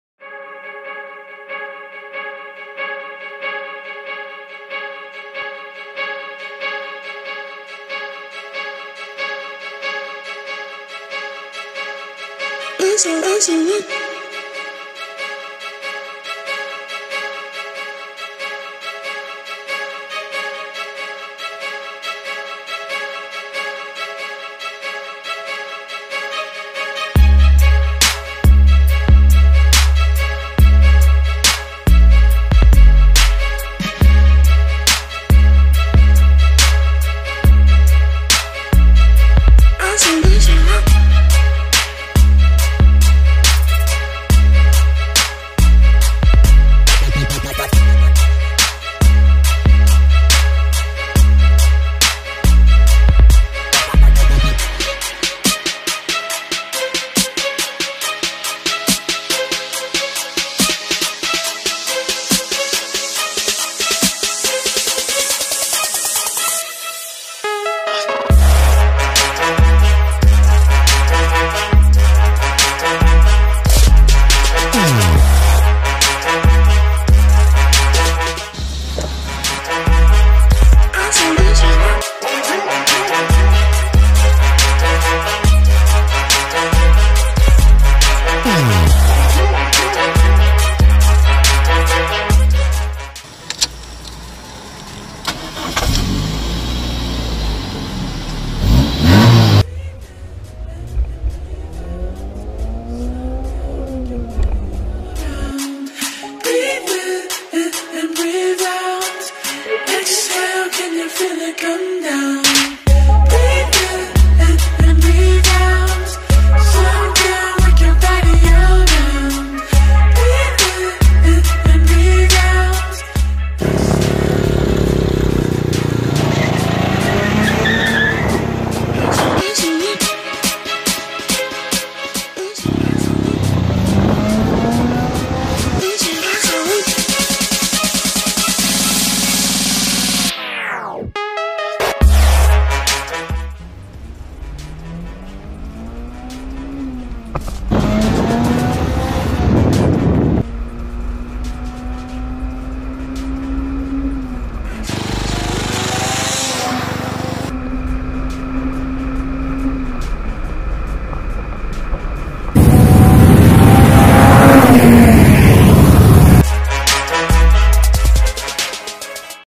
AE111 factory GT | SRINTER | JDM | Bangladesh
presenting one of the cleanest ae111 sprinter gt and its capability
Watch full and use earphone for better experience